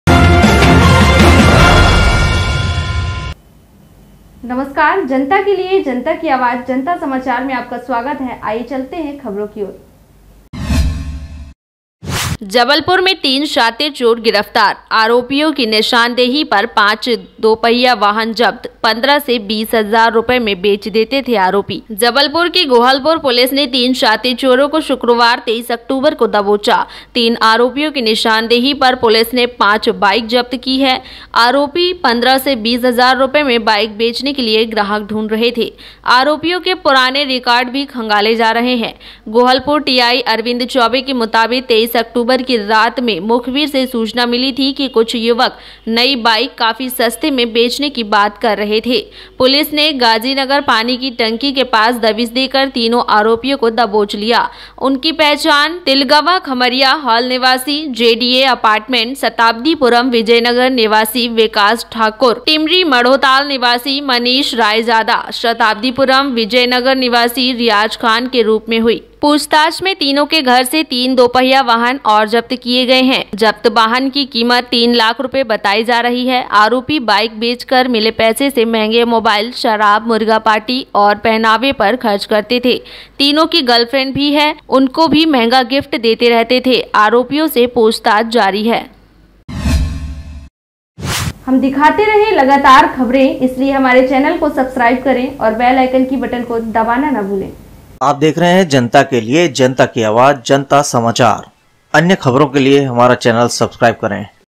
तीन शातिर चोर गिरफ्तार Jabalpur, 23/10/2021 JantaSamachar.News#HindiNewsChannel
NewsInHindiSamachar,
Janta samachar,
three vicious thieves arrested,
तीन शातिर चोर गिरफ्तार,

जबलपुर की गोहलपुर पुलिस ने तीन शातिर चोरों को शुक्रवार 23 अक्टूबर को दबोचा है। तीनों आरोपियों की निशानदेही पर पुलिस ने पांच बाइक जब्त किए हैं। आरोपी 15 से 20 हजार रुपए में बाइक बेचने के लिए ग्राहक ढूंढ रहे थे। आरोपियों के पुराने रिकॉर्ड भी खंगाले जा रहे हैं।

गोहलपुर टीआई अरविंद चौबे के मुताबिक 23 अक्टूबर की रात में मुखबिर से सूचना मिली थी कि कुछ युवक नई बाइक काफी सस्ते में बेचने की बात कर रहे थे। पुलिस ने गाजी नगर पानी की टंकी के पास दबिश देकर तीनों आरोपियों को दबोच लिया। उनकी पहचान तिलगवां खमरिया हाल निवासी जेडीए अपार्टमेंट शताब्दीपुरम विजय नगर निवासी विकास ठाकुर (20), टिमरी माढ़ोताल निवासी मनीष रायजादा (श्रीवास्तव) (22), शताब्दीपुरम विजय नगर निवासी रियाज खान (31) के रूप में हुई।

पूछताछ में तीनों के घर से तीन दोपहिया वाहन और जब्त किए गए। आरोपियों ने एमपी 20 केएच 7697 को 15 दिन पूर्व शैल्वी अस्पताल के सामने से, एमपी 20 एसएम 6662 को एक साल पहले शताब्दीपुरम से और वाहन बिना नंबर की 2 माह पहले गोलबाजार से चुराया था। जब्त वाहन की कीमत तीन लाख रुपए बताई जा रही है। आरोपी बाइक बेच कर मिले पैसे से महंगे मोबाइल, शराब-मुर्गा पार्टी और पहनावे पर खर्च करते थे। तीनों की गर्लफ्रेंड भी है। उनके भी महंगा गिफ्ट देते रहते थे। आरोपियों पूछताछ जारी है।

The Gohalpur Police of Jabalpur has arrested three vicious thieves on Friday, 23 October. On the spot of the three accused, the police have seized five bikes. The accused were looking for customers to sell the bike for 15 to 20 thousand rupees. The old records of the accused are also being searched.

According to Gohalpur TI Arvind Choubey, on the night of 23 October, information was received from the informer that some youths were talking about selling new bikes very cheaply. Police arrested the three accused by raiding near Ghazi Nagar water tank. They were identified as Vikas Thakur (20), resident of JDA apartment Shatabdipuram Vijay Nagar, resident of Tilgwan Khamaria Hall, Manish Raizada (Srivastava) (22), resident of Timri Madhotal, Riyaz Khan (31), resident of Shatabdipuram Vijay Nagar.

During interrogation, three more two-wheelers were seized from the house of the three. The accused had stolen MP 20 KH 7697 from opposite Shelvi Hospital 15 days back, MP 20 SM 6662 from Shatabdipuram a year ago and vehicle without number from Golbazar 2 months ago. The value of the seized vehicle is said to be Rs. The accused used to spend on expensive mobiles, liquor-cock parties and clothes from the money received by selling bikes. All three also have girlfriends. He also used to give expensive gifts. The interrogation of the accused is on.